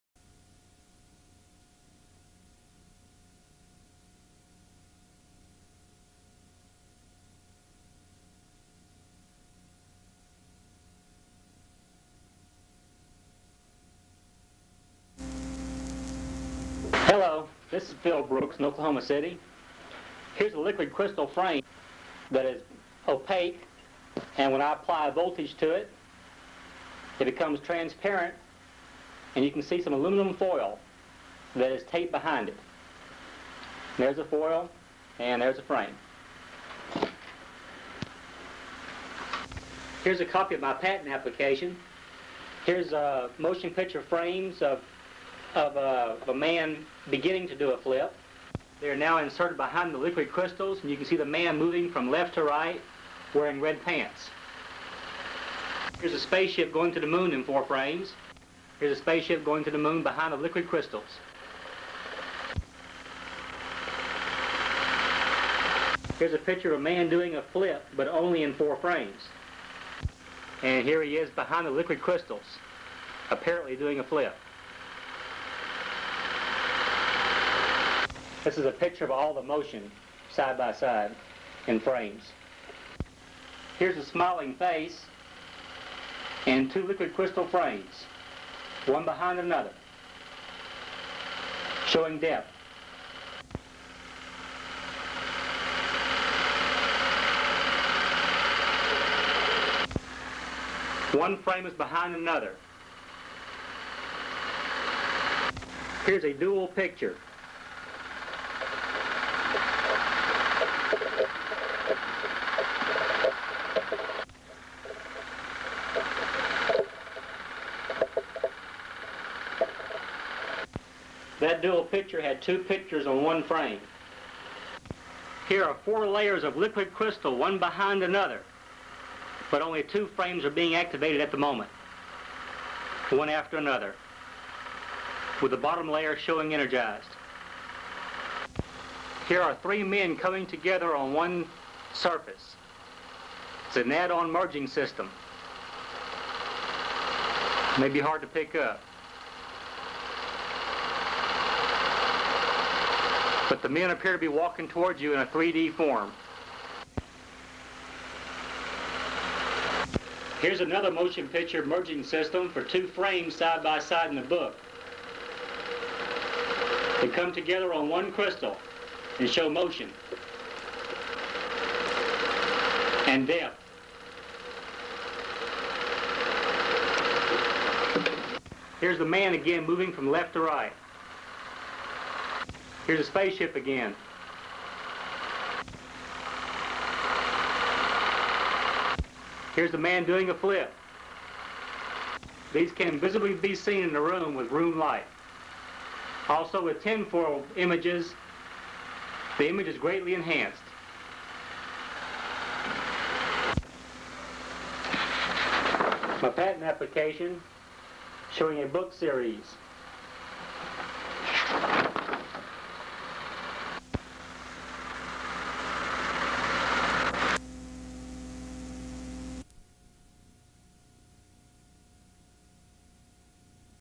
Phil Brooks Invention Film
A Film documenting my dads inventions